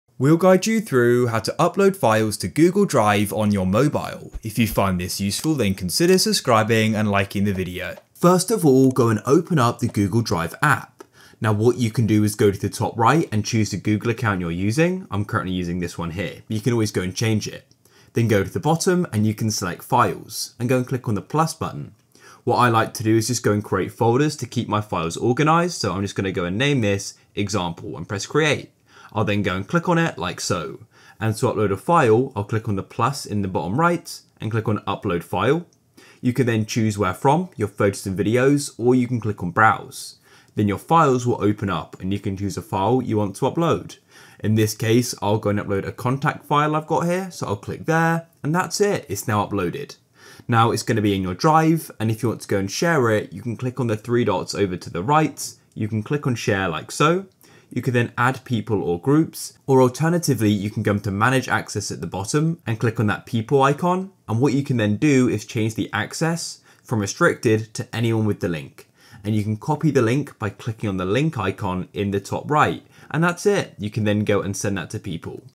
How To Upload Files On Google Drive On Mobile - Full Guide
Learn how to upload files on google drive on mobile in this video.

GuideRealm